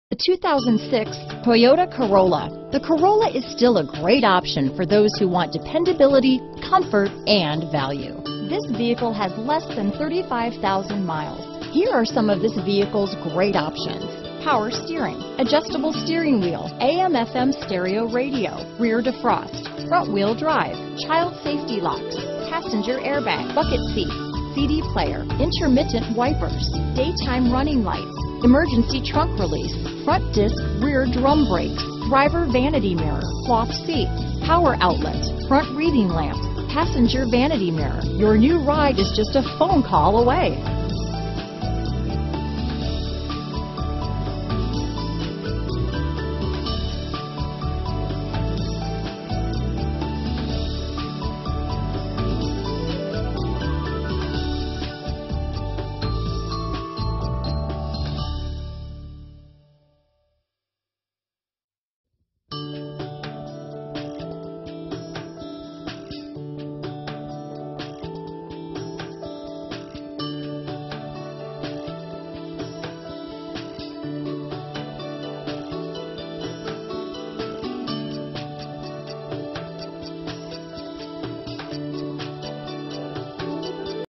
2006 Toyota Corolla Waukegan, Gurnee, Kenosha, WI, Fox Lake, Libertyville, IL T17377B
2006 Toyota Corolla

Classic Toyota
515 N Green Bay Rd

*Clean* *Low Miles* *CE* Yes the miles are this low! Check out this automatic 2006 Toyota Corolla CE! This Corolla is the definition of a reliable vehicle. If you are looking for an inexpensive reliable vehicle, you just found it. This one will NOT last long. Visit Classic Toyota Today! The Friendliest Place In Town!